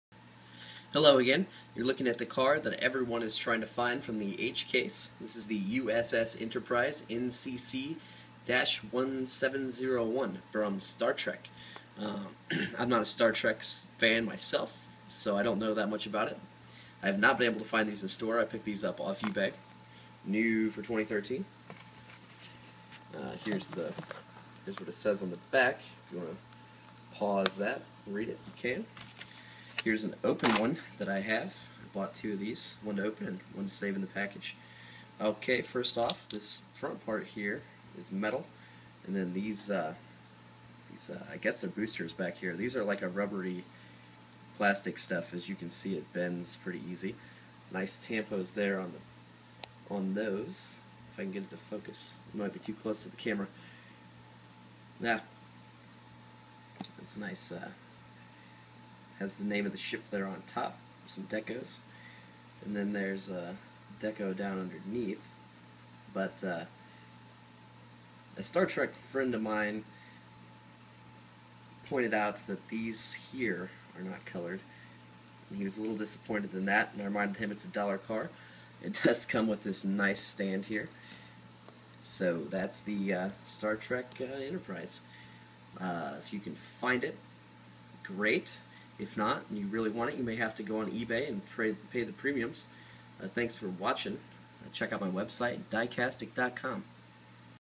Hot Wheels Star Trek USS Enterprise NCC 1701
In this video I review the hard to find Hot Wheels Star Trek USS Enterprise NCC 1701 from the 2013 H case.

eBay.com/GranaToys
GranaToys.com

See photos and exclusive stories:
Diecastic.com